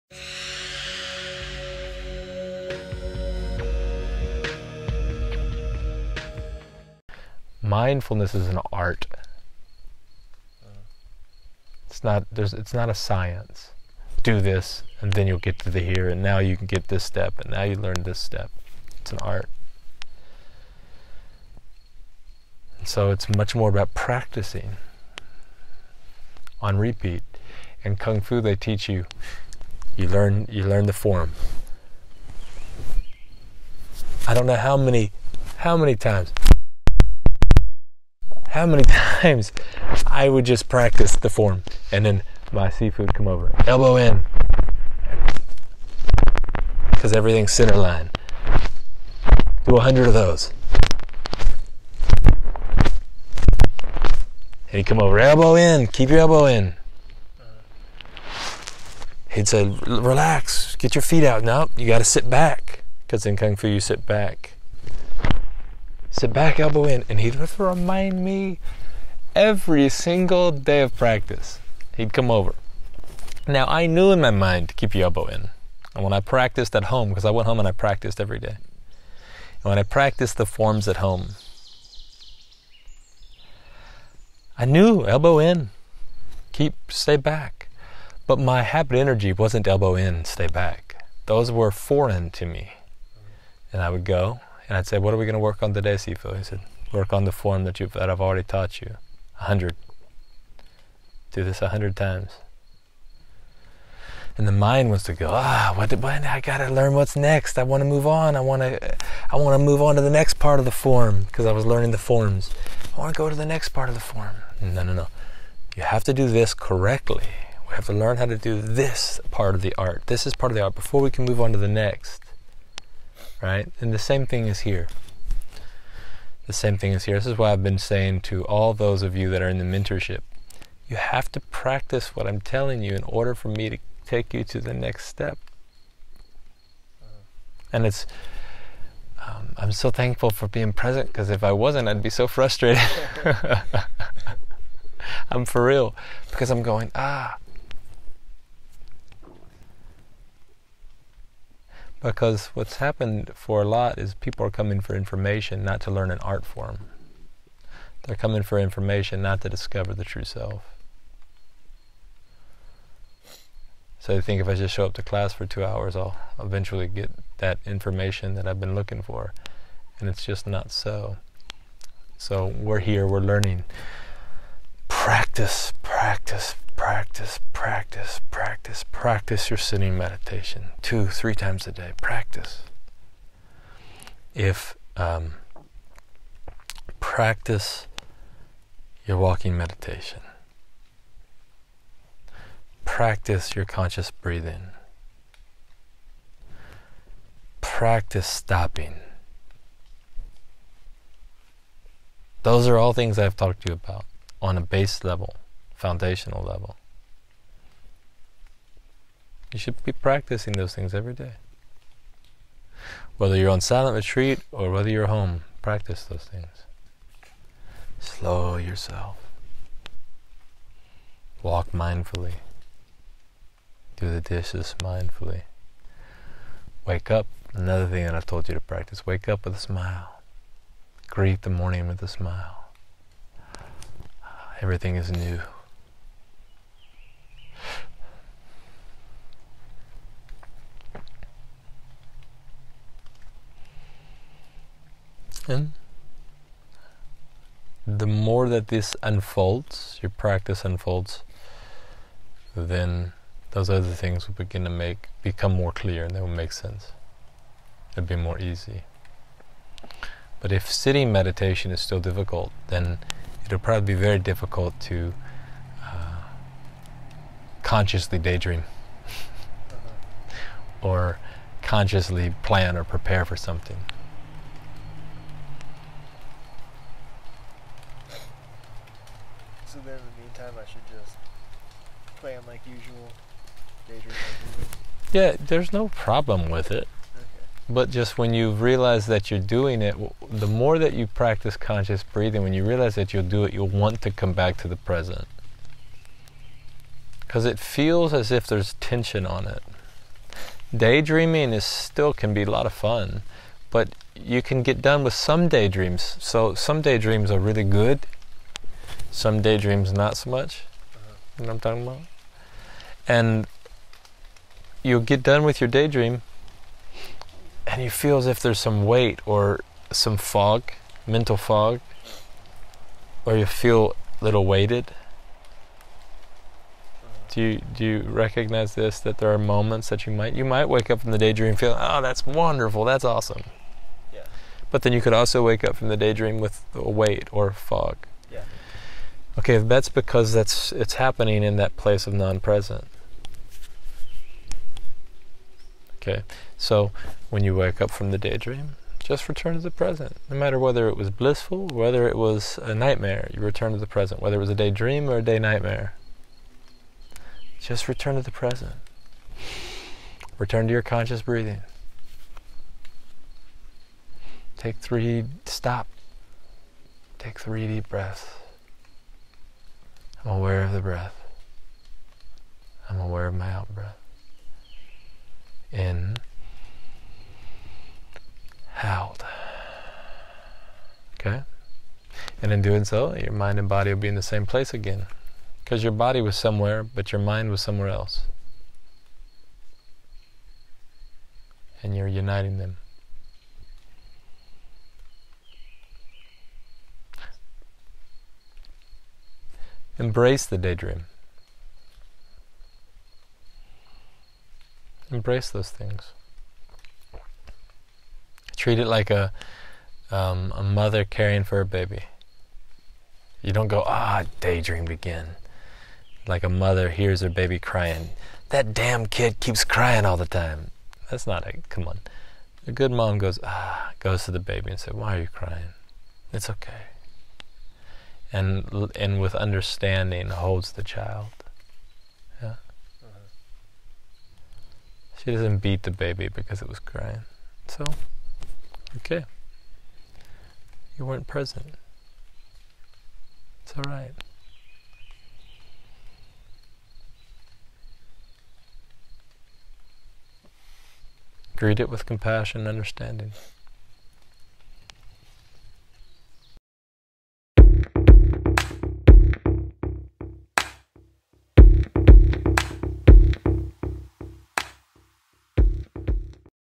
The Art Of Mindfulness: How To Overcome Distractions
Mindfulness is an art form, not a science. True students give themselves entirely to the practice of this beautiful art.  They often begin practice looking for enlightenment but if they are patient and committed they will discover something far beyond their idea of enlightenment...they will discover peace of mind.